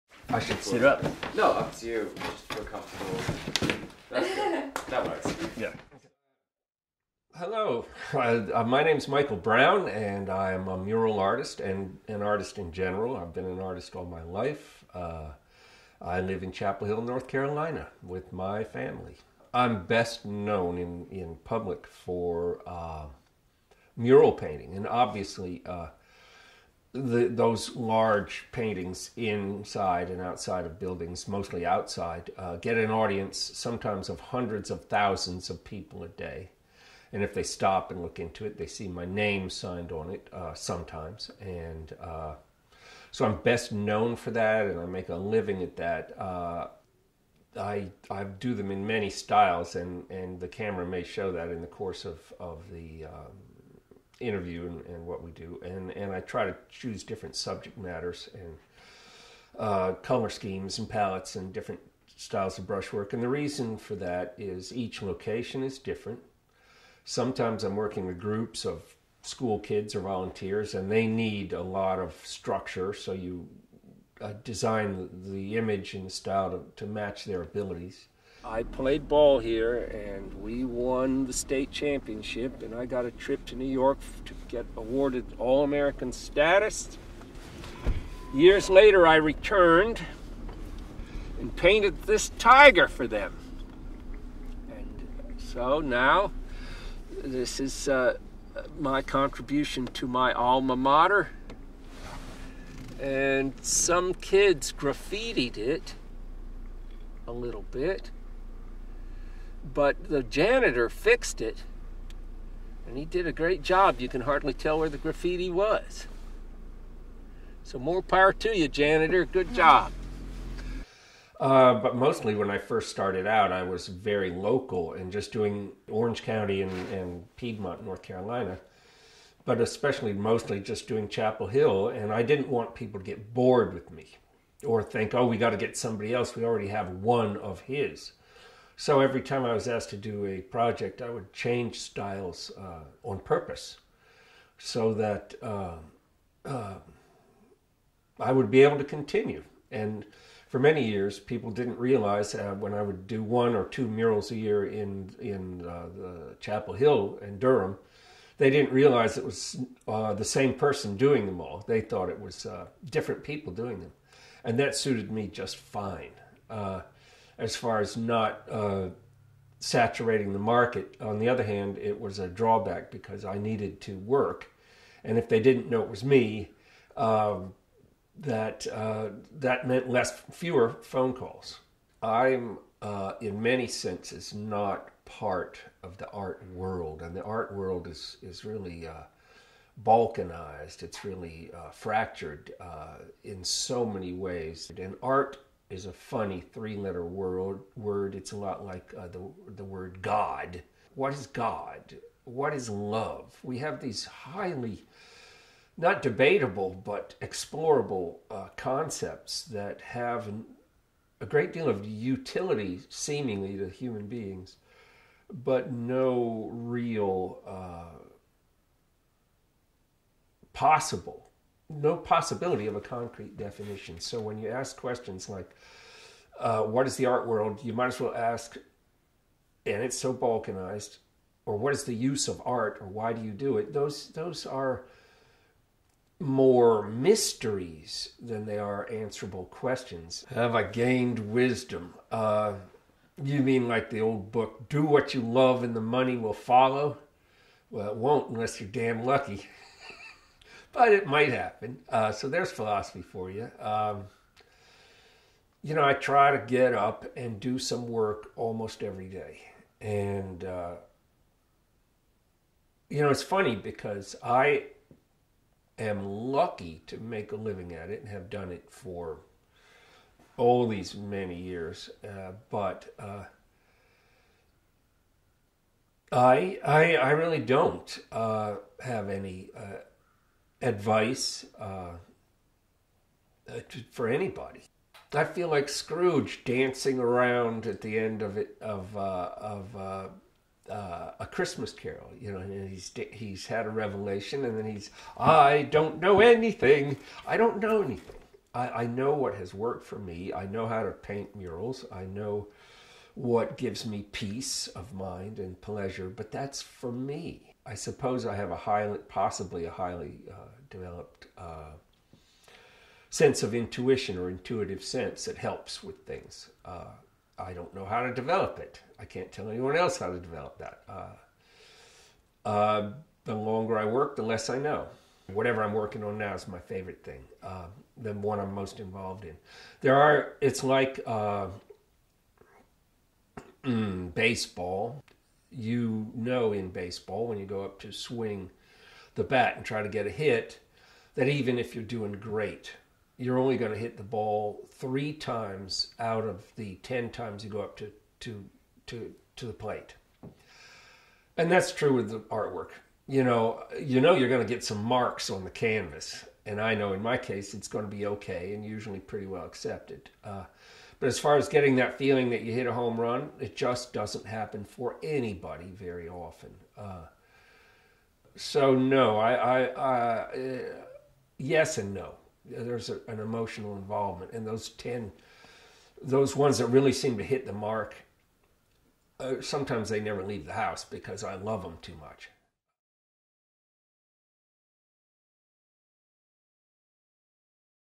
Michael Brown (2016)
a short documentary on what local Chapel Hill mural artist, Michael Brown, thinks of art. He delves in to his journey on how he became an artist and talks about how he feels about the art world.